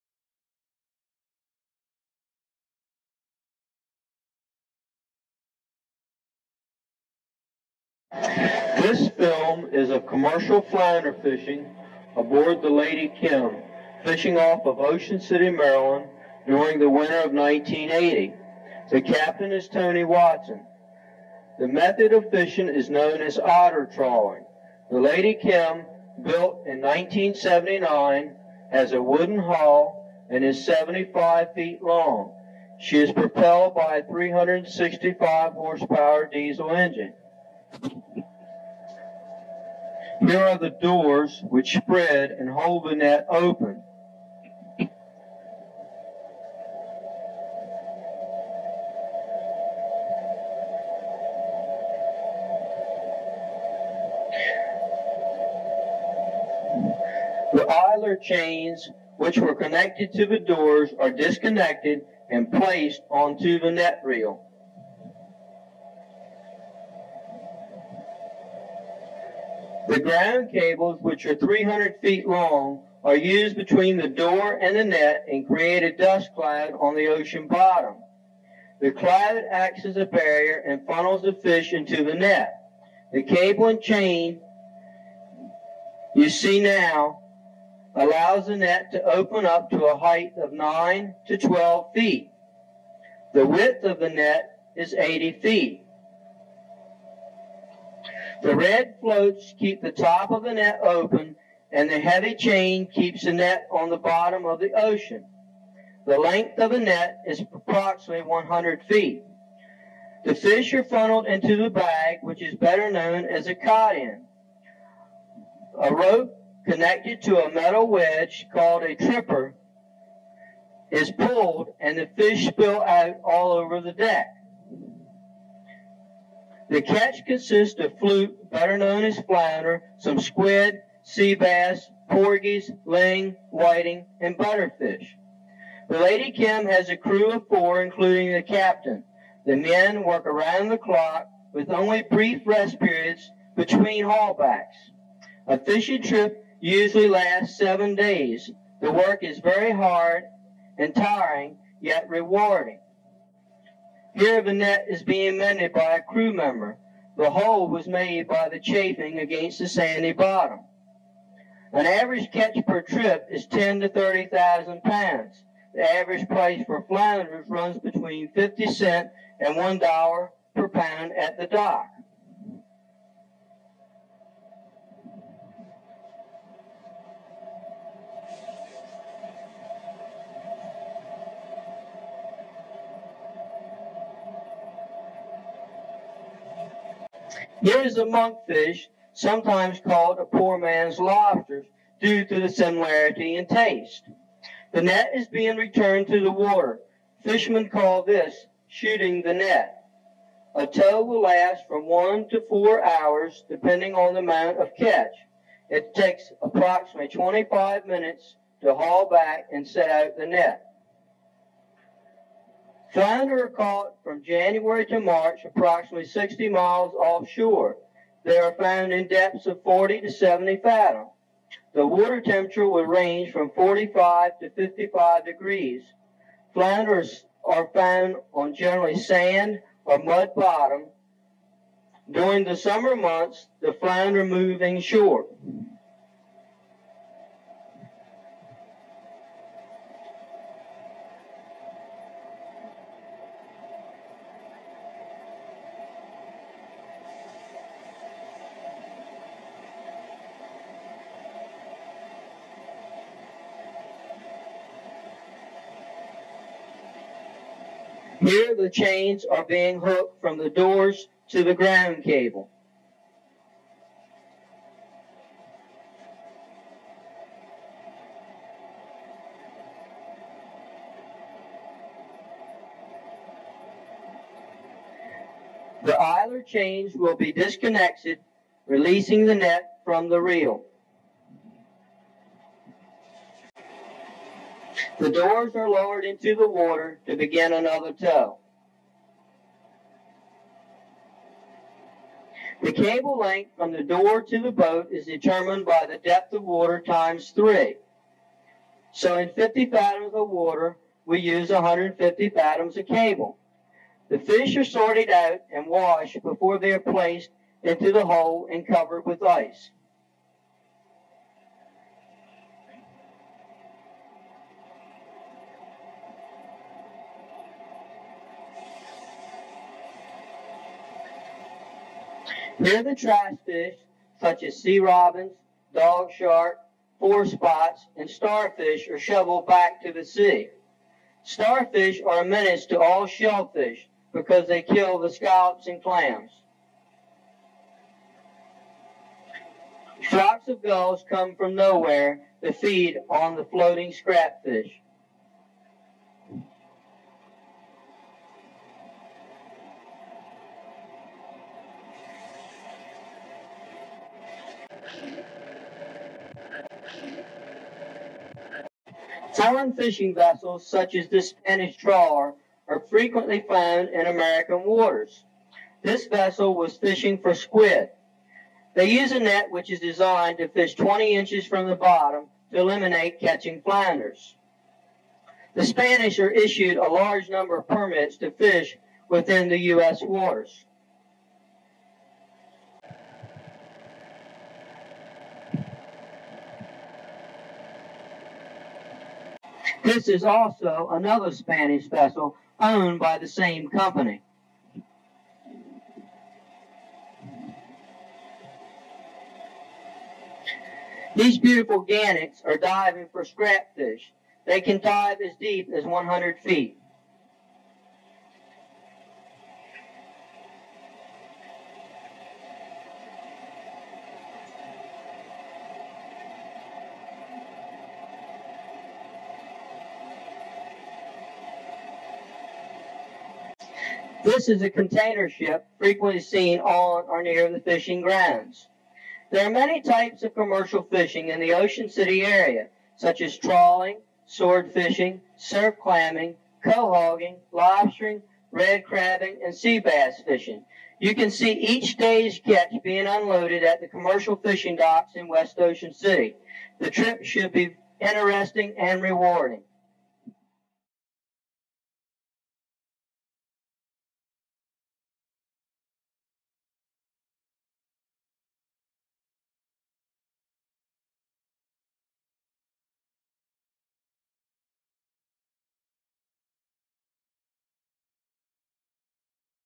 Commercial Fishing, Winter 1980/Ocean City, Maryland
Marsh Video Productions digitized this Super 8mm film with sound for the Ocean City Life-Saving Station Museum. Very interesting to see how commercial fishing was done forty years ago.